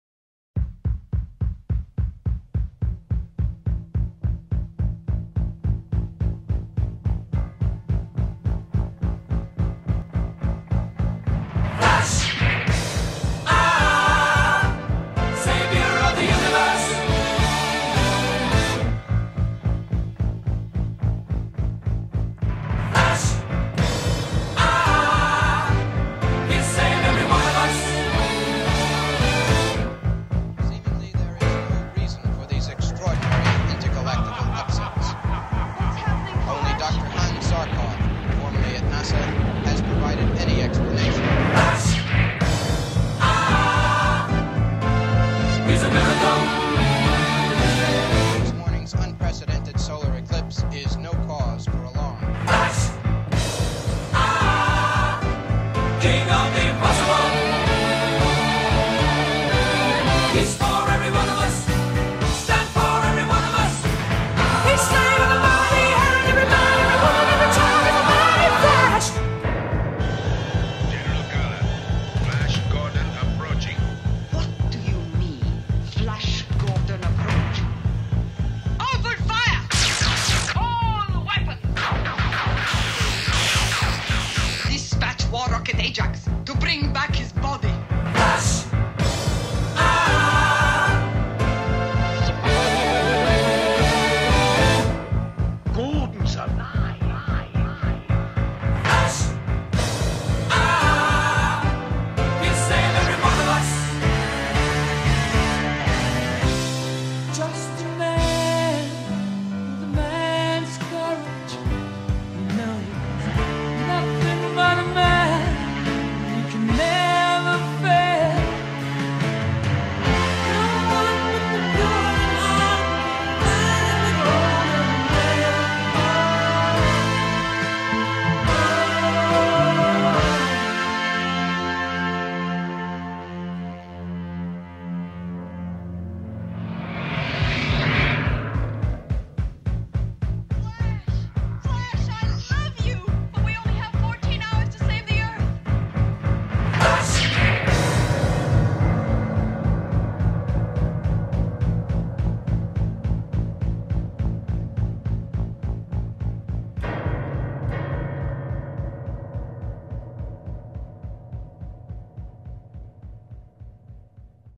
Flash
Here is a short funny video we made of Flash. our little naked Red Squirrel. Enjoy.